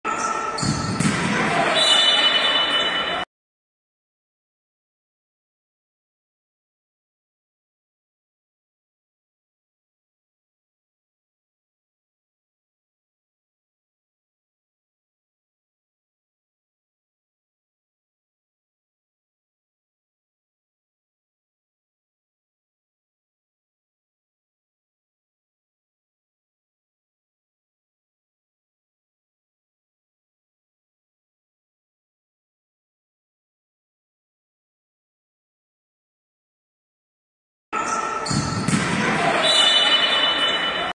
Video 1281 Tiro Libre Directo y Expulsión
Si un jugador se emplea o tiene la intención de emplearse con fuerza excesiva o con brutalidad contra un adversario cuando no le está disputando el balón, o contra cualquier otra persona, independientemente de si se produce o no contacto, la acción será considerada conducta violenta. La conducta violenta puede ocurrir dentro o fuera del terreno de juego, estando o no el balón en juego. No se concederá ventaja en el caso de juego brusco y grave (falta de extrema dureza), conducta violenta, faltas merecedoras de una segunda amonestación o tras la sexta y sucesivas faltas acumulables, a no ser que se trate de una ocasión manifiesta de gol y dadas las consideraciones no existe motivo ante esta acción en otorgar una ventaja.